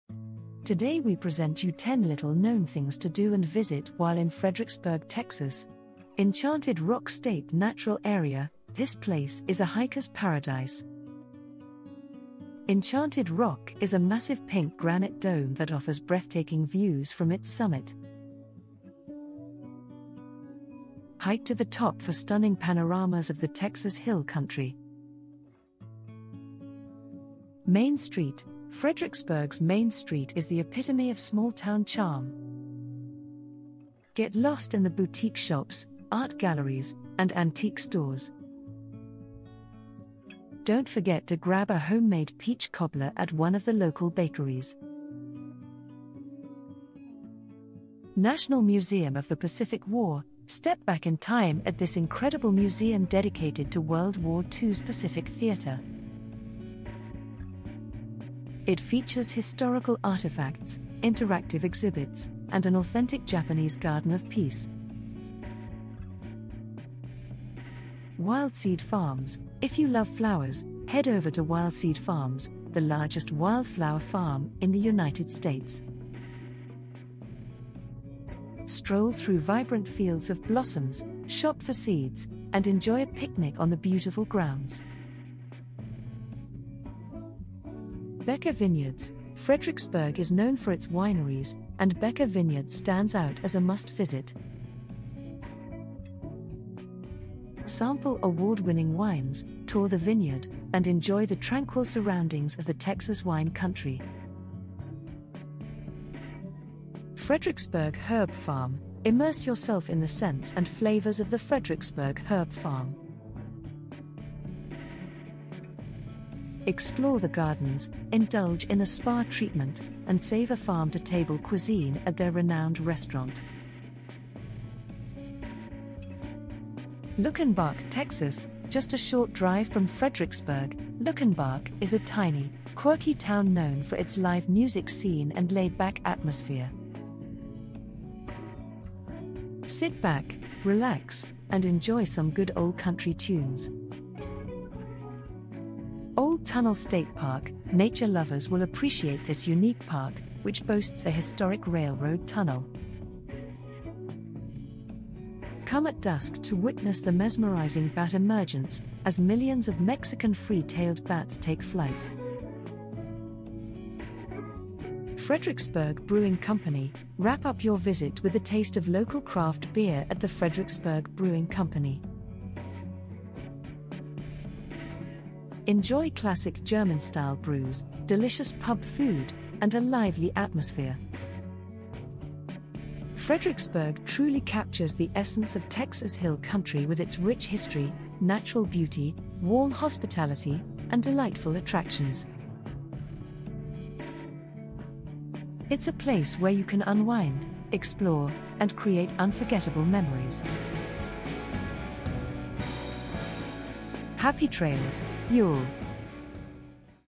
9 Little Know Things To Do and Places To Visit While in Fredericksburg Texas
9 Little Know Things To Do and Places To Visit While in Fredericksburg Texas. In this captivating video, we bring you an insider's guide to the hidden gems of Fredericksburg, Texas. While this quaint town is known for its rich history and German heritage, we are here to unveil the nine lesser-known things to do and visit during your stay. From exploring enchanting vineyards and indulging in locally-produced wine to discovering charming boutiques and art galleries tucked away in the charming streets, we have curated a list that will surely surprise you. Immerse yourself in the beauty of nature at the serene Enchanted Rock State Natural Area or embark on a culinary adventure by savoring authentic Texan barbecue and delicious German cuisine. We have also included some off-the-beaten-path spots that will leave you awe-inspired. So, if you're looking for a unique and unforgettable experience in Fredericksburg, join us in this virtual journey, where we unveil the nine little-known things to do and visit that will make your trip truly extraordinary.